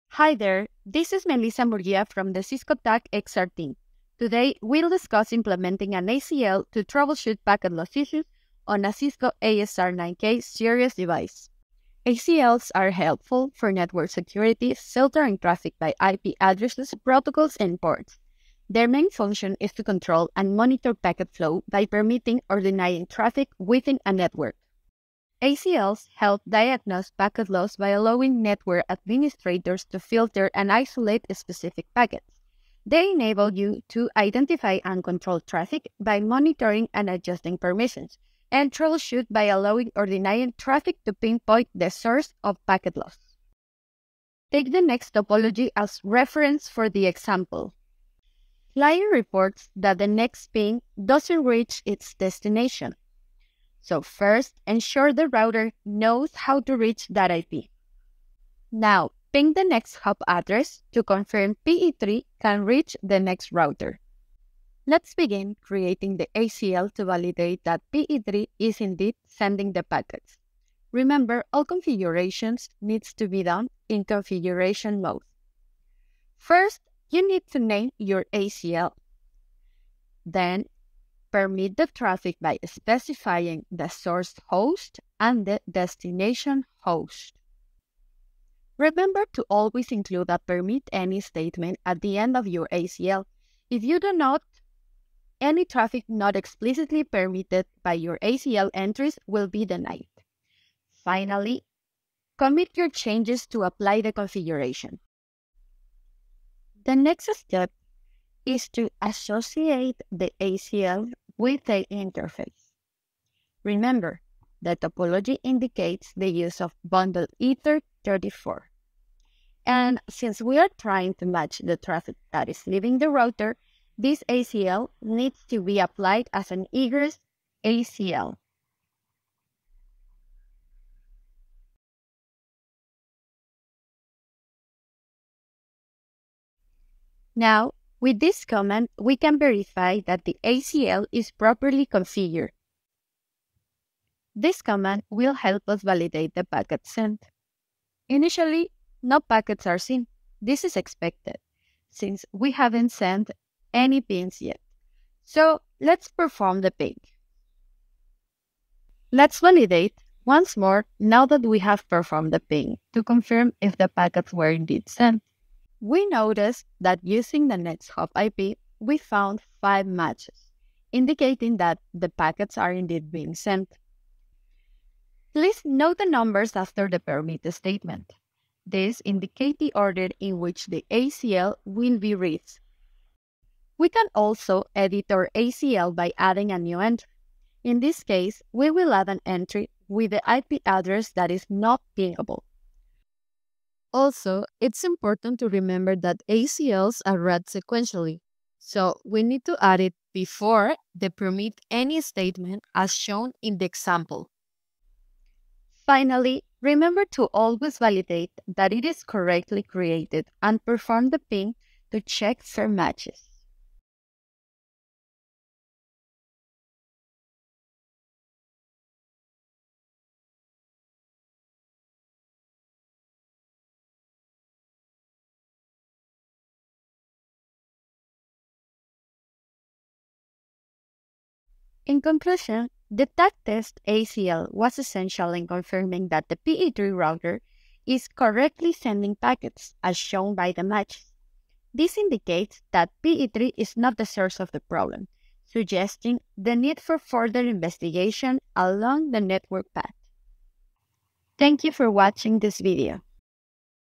Configure Access Lists to Troubleshoot Packet Loss | Network Visibility and Control
This video shows you how to use access lists (ACLs) to troubleshoot packet loss in your Cisco network. Learn how to filter traffic, pinpoint dropped packets, and gather insights into what’s causing disruptions—so you can quickly restore performance and improve reliability.

 What you’ll learn in this video:
  – How to configure standard and extended ACLs to monitor traffic
  – Techniques to isolate and identify packet loss issues
  – Best practices for using ACLs to improve network diagnostics